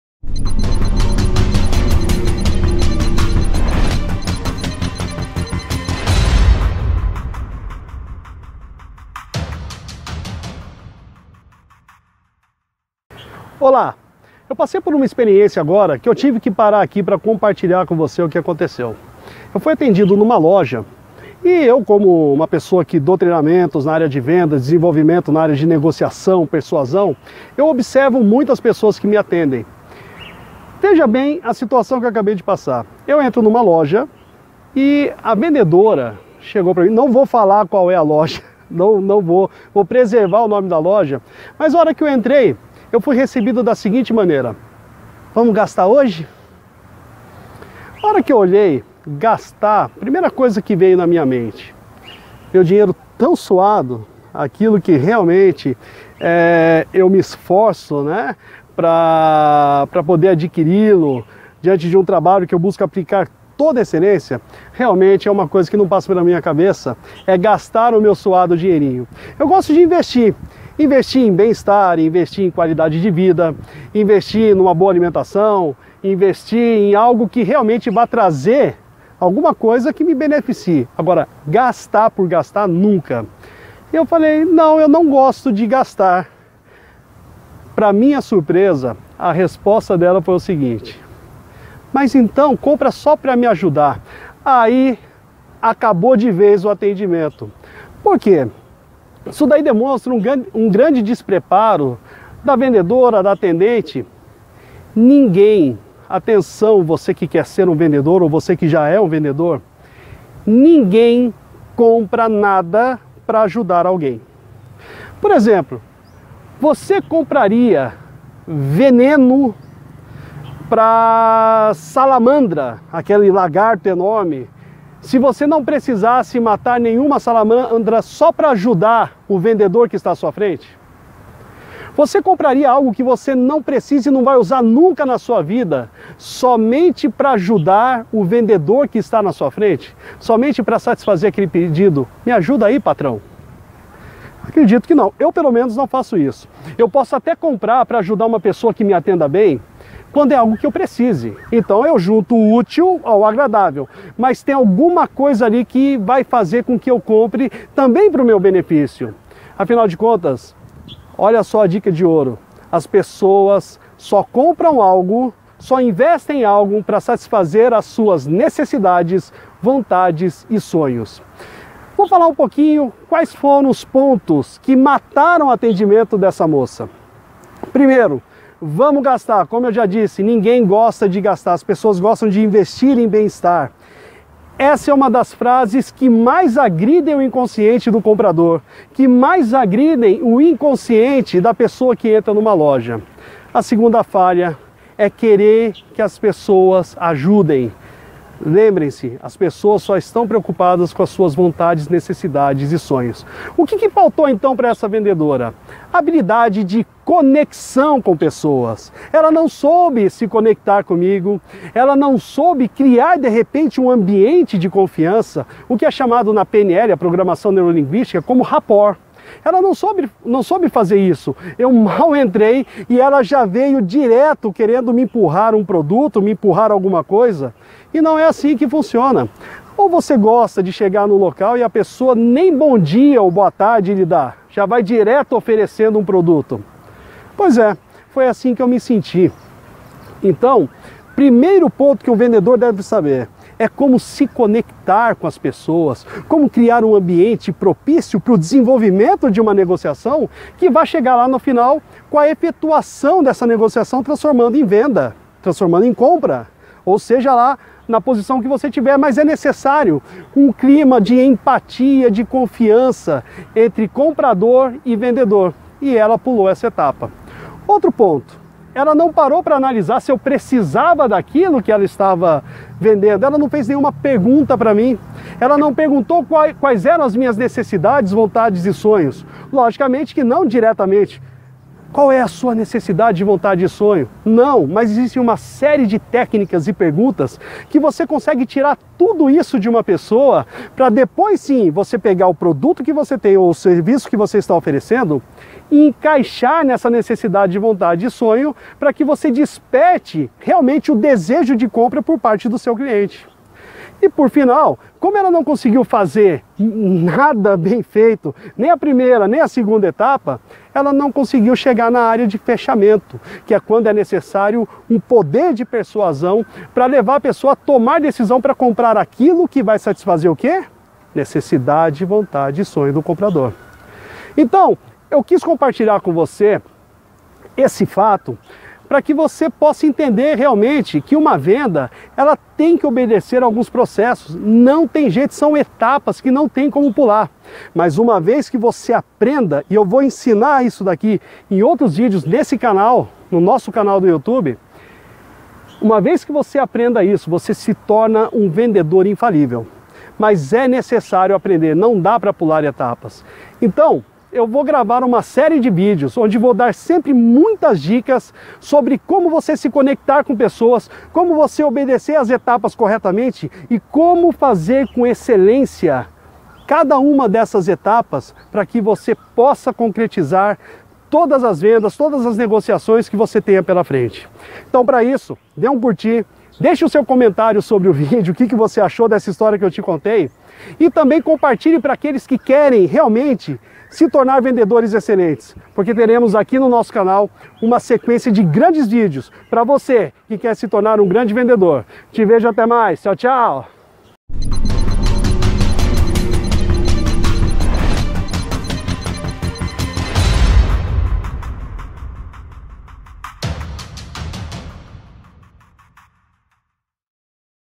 Eu não comprei, você compraria ?
Nesse vídeo, eu conto uma história que aconteceu comigo, quando fui atendido em uma loja. Perceba que são gafes que muitos cometem e não sabem.

Quer vender mais ?

Então fuja de gafes como essa para atender melhor ao seu cliente...